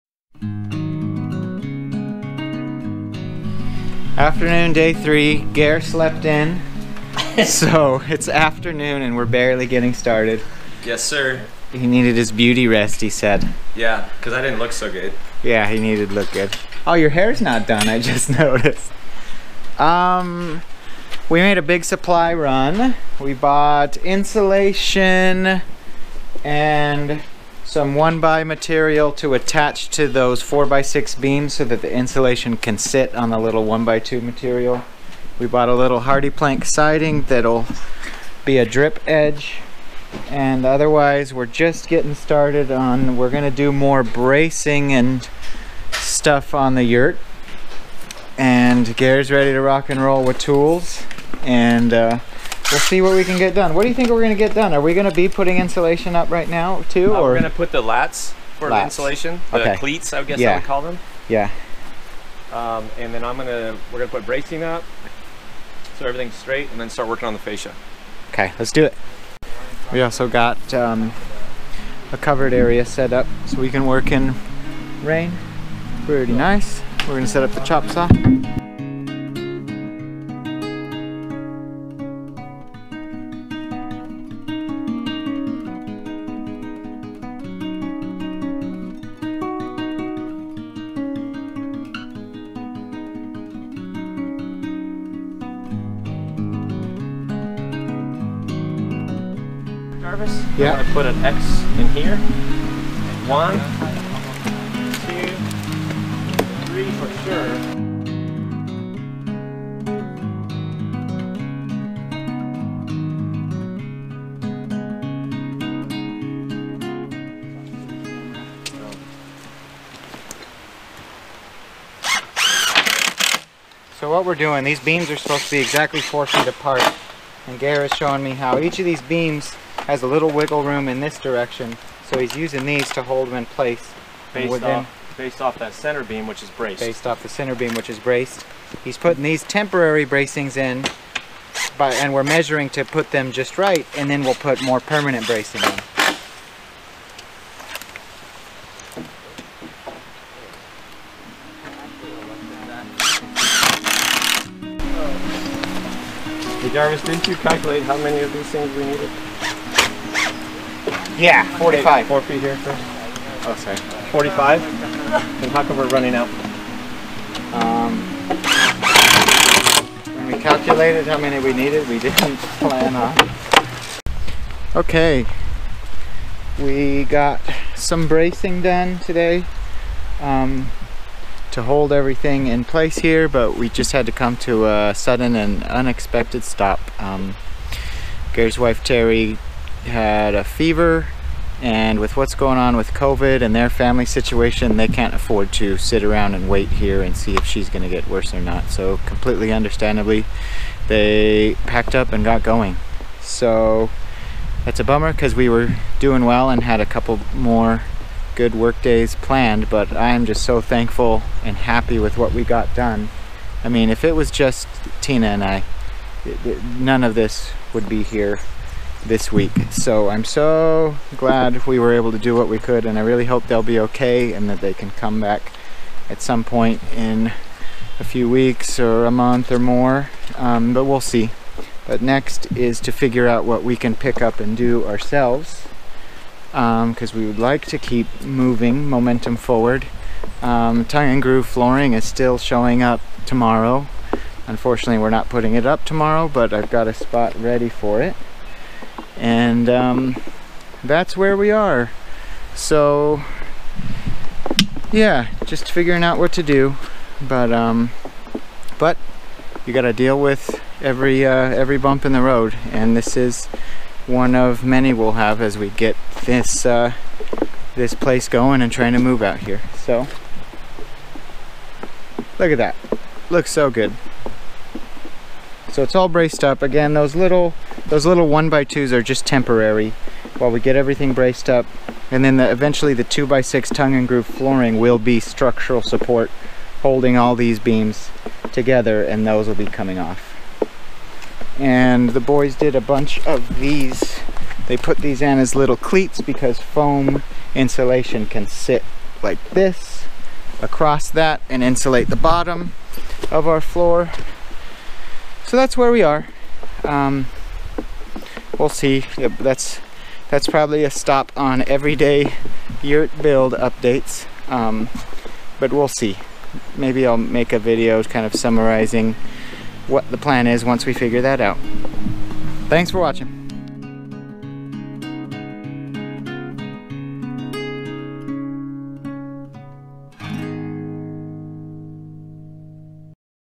Building a Yurt Foundation, Day 3 - Bracing and a Pause On Work
We started the day with a supply run, buying foam insulation and 1x2 material.  We got bracing done to support the foundation, but then had to come to an unexpected stop.  Hoping to pick up the forward momentum again soon.